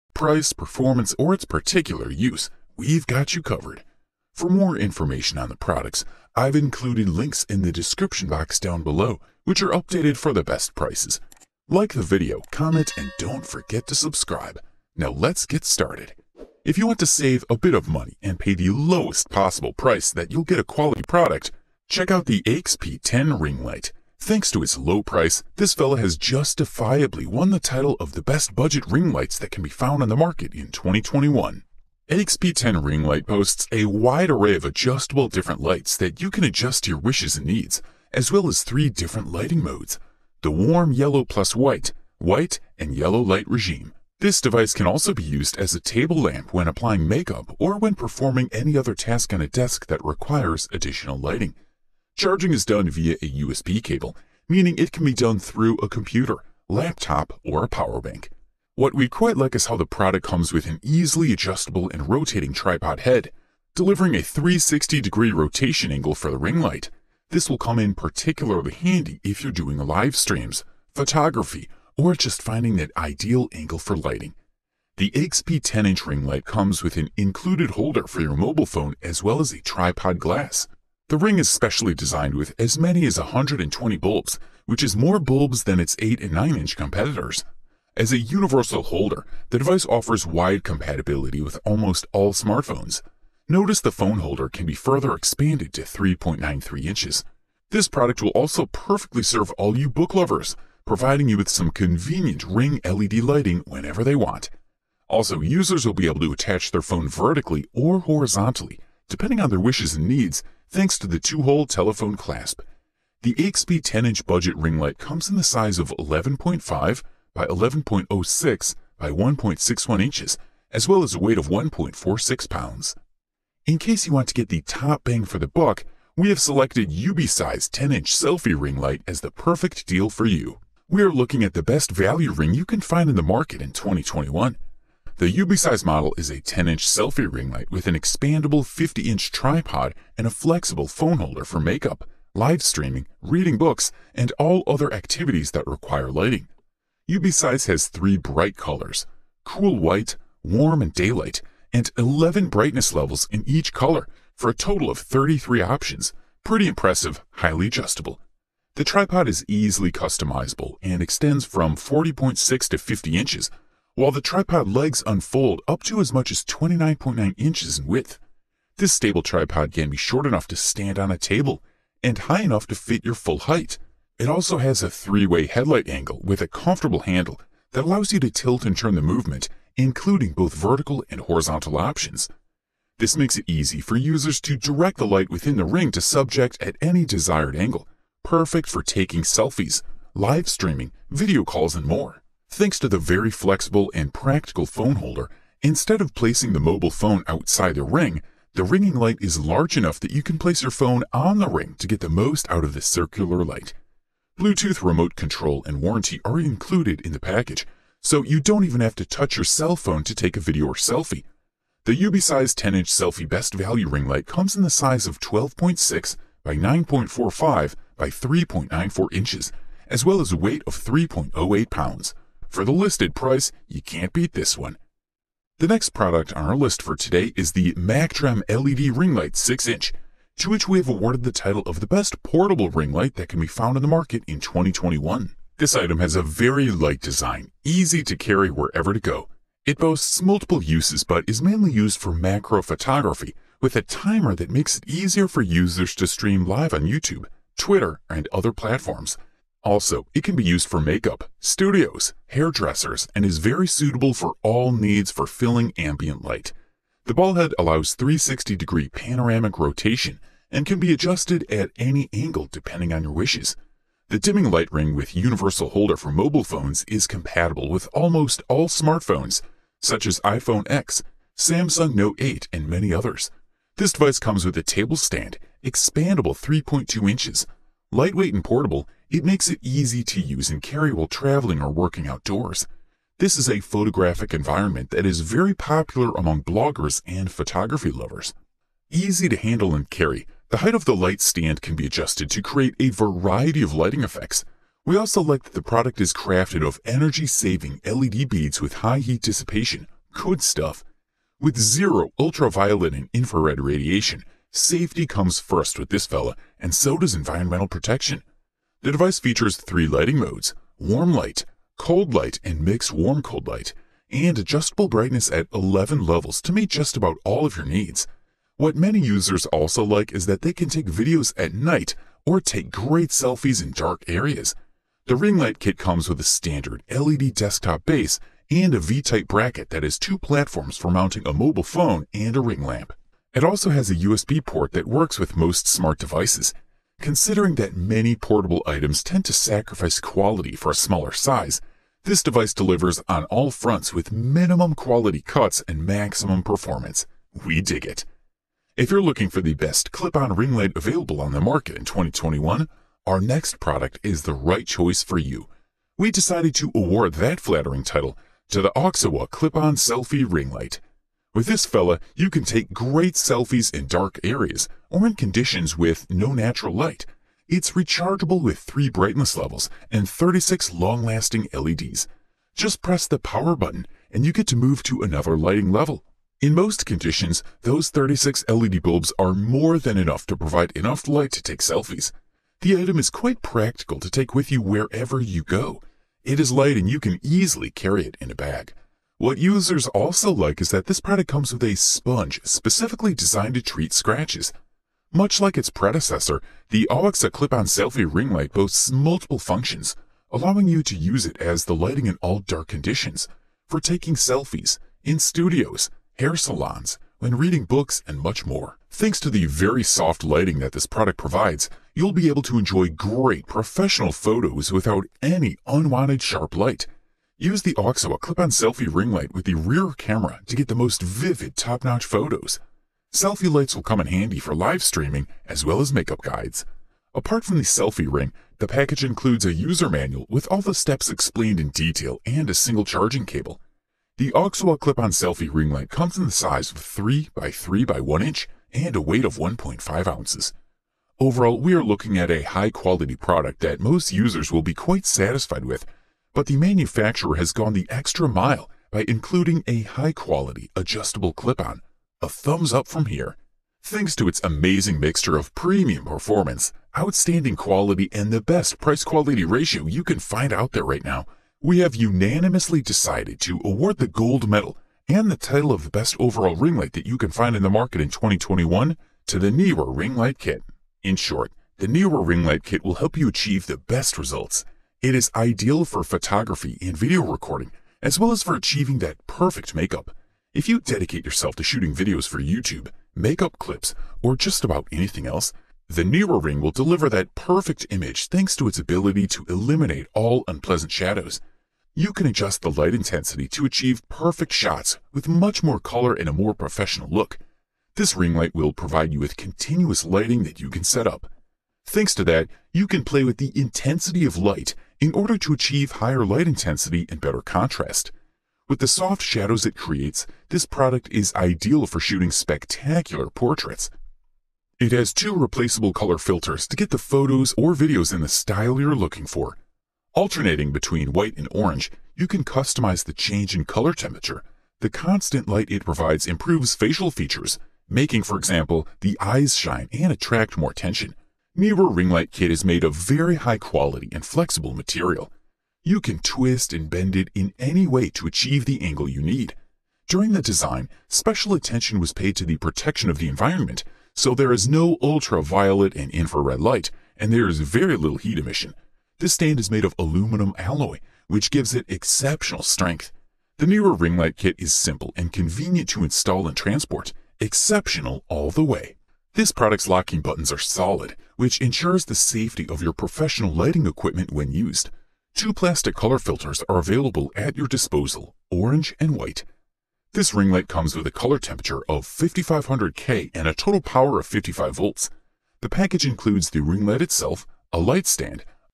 BEST Ring Lights and Selfie Stands-Muson LED Selfie-AccLoo Ring-Eocean 10.2in Ring-Flexible Tripod
BEST Ring Lights and Selfie Stands

➜ Links to the Best Ring Lights 2022 listed in this video:

►US Links◄
➜ Muson LED Selfie
➜AccLoo Ring
➜Eocean 10.2in Ring
➜Flexible Tripod

We have just laid out the  best ring lights and stands . In 5th place is the AIXPI 10” Ring Light, our pick for the best budget ring light. In 4th place is the UBeesize 10” Selfie Ring Light, our pick for the best value ring light. In 3rd place is the MACTREM LED Ring Light 6”, our pick for the best portable ring light. In 2nd place is the Auxiwa Clip On Selfie Ring Light, our pick for the best clip on ring light. In 1st place is the Neewer Ring Light Kit, our pick for the best overall ring light.